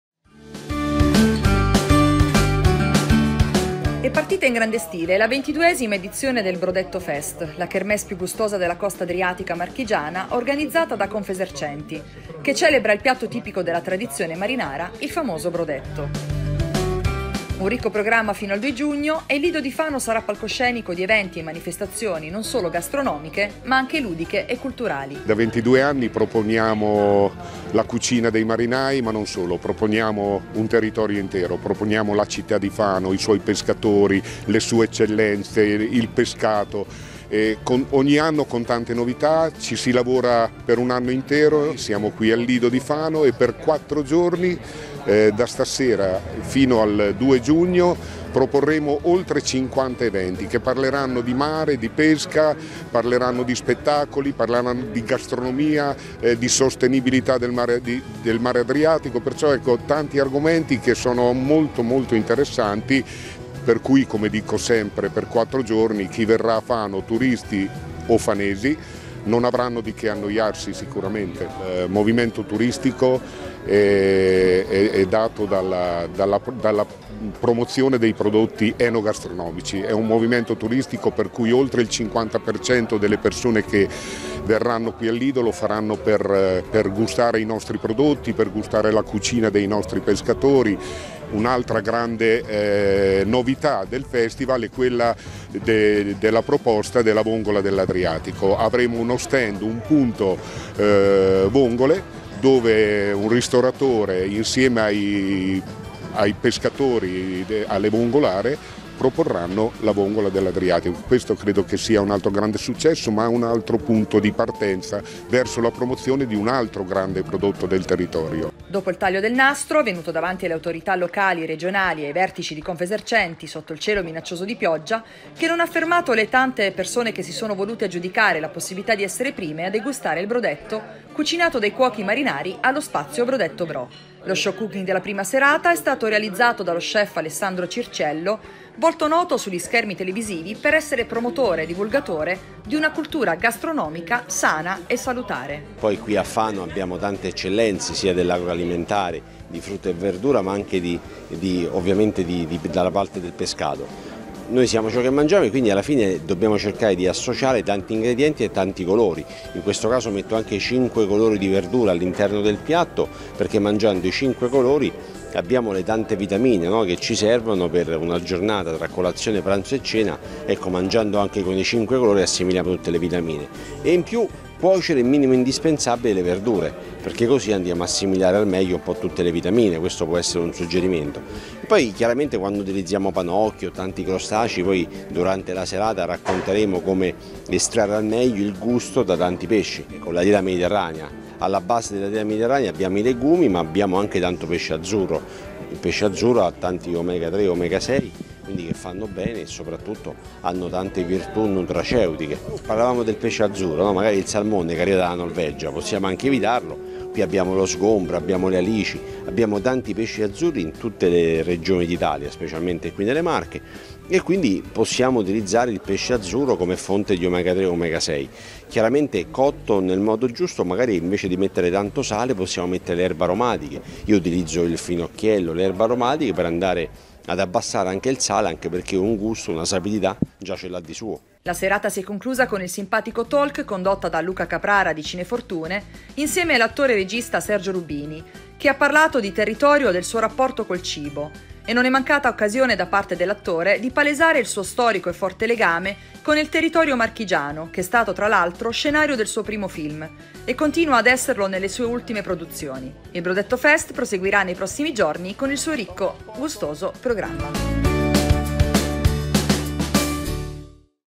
Brodettofest: fino a Domenica al lido di Fano
Al Lido di Fano è entrato nel vivo il BrodettFest: il festival internazionale dei brodetti e delle zuppe di pesce ideato da Confesercenti di Pesaro e Urbino.